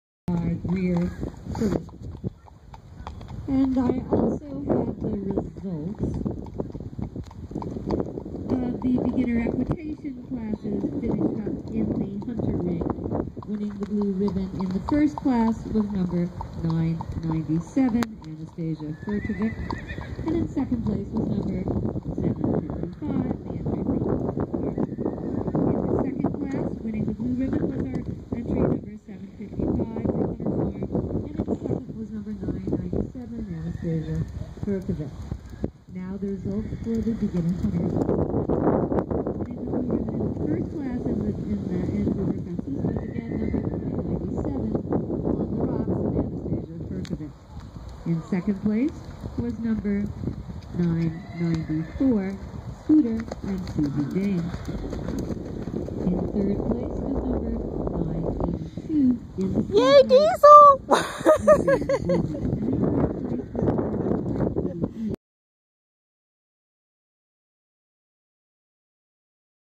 Diesel at The  Ridge. Level 0 jumpers  ridden by Grier Filley